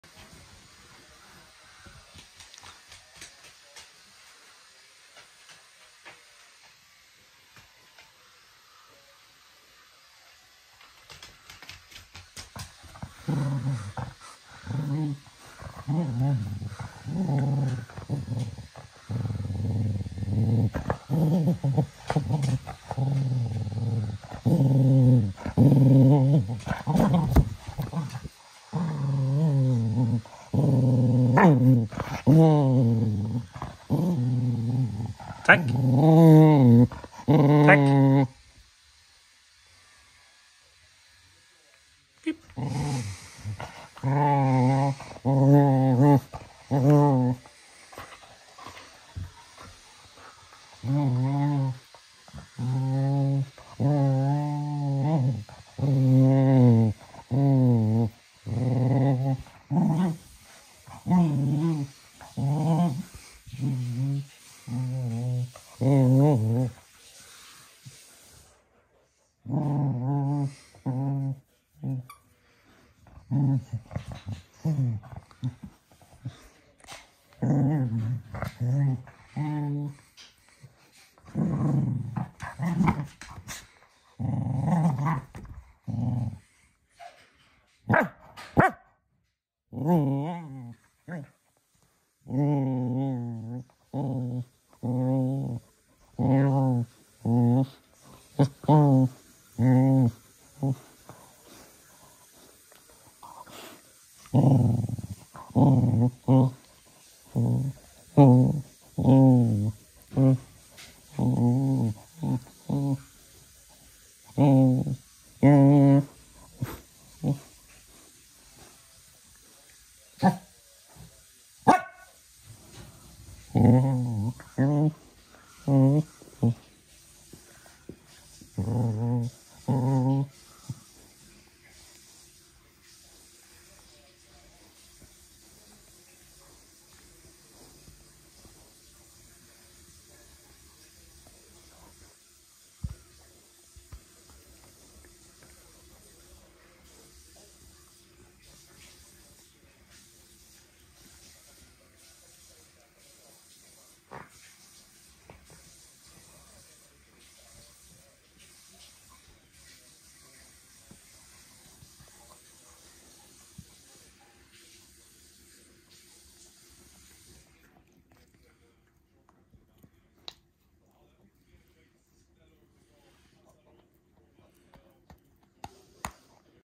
Evening playtime for our little cocker spaniel
Manfred has the most energy when it’s time for bed, this morning when taking him for a run at 6 in the morning I had to drag him with me. At 10 in the evening he has way more energy 🥰